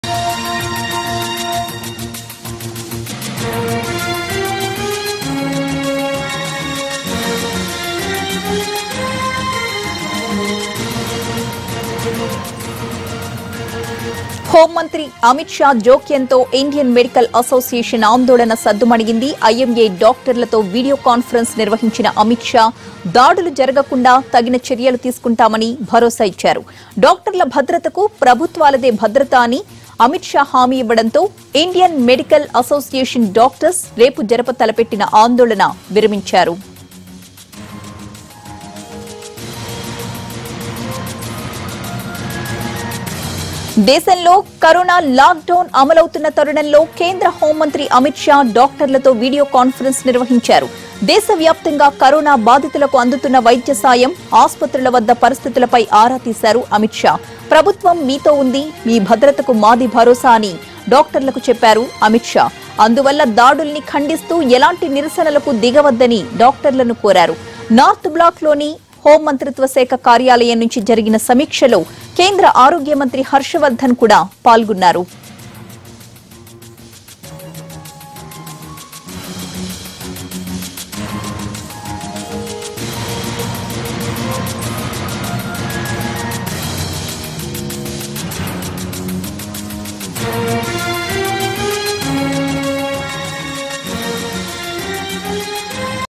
IMA Doctors Withdraw Black Day Protest After Video Conference With Amit Shah || NTV
IMA Doctors Withdraw Black Day Protest After Video Conference With Amit Shah